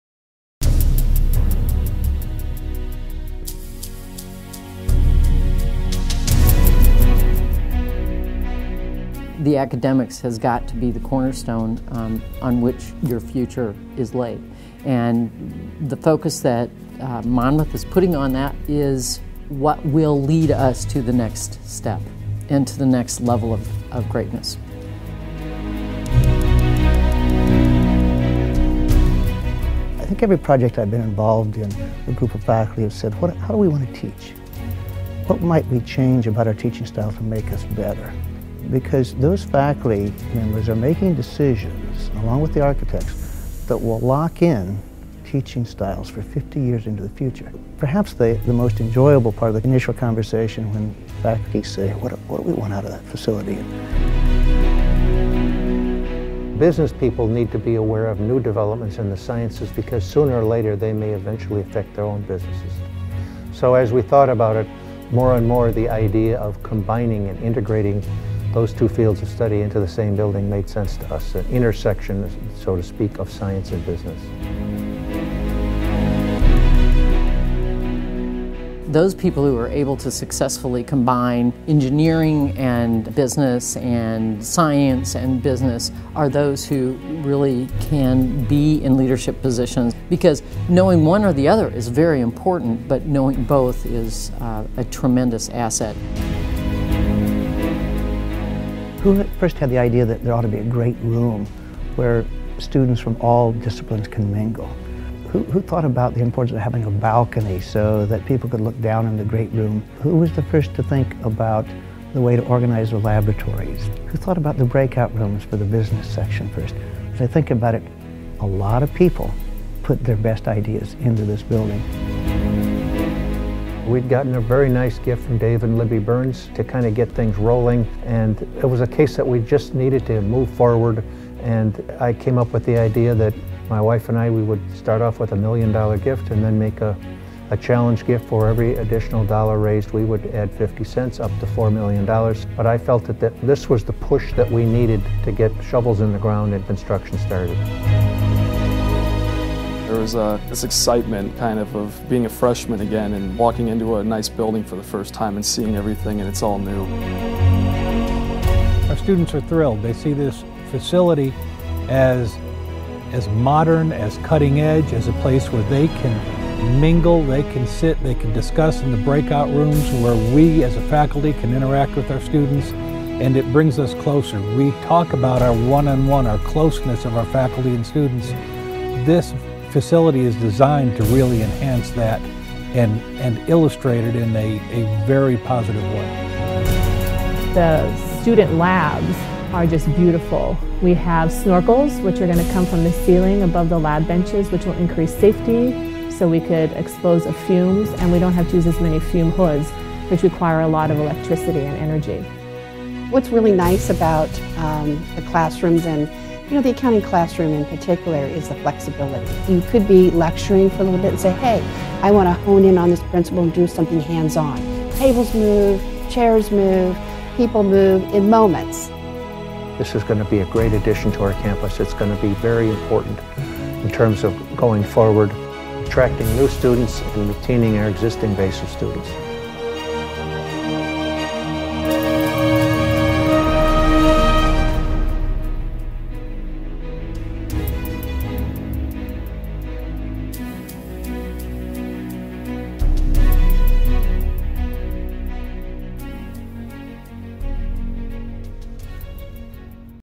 A New Academic Era: Center for Science & Business -- Monmouth College
Trustees, faculty and students discuss the academic impact of Monmouth College's new Center for Science and Business. May 2013.

The unique $42 million building combines mathematics and the sciences with business, economics, and accounting so students can be more marketable and can lead inter-disciplinary efforts after graduation.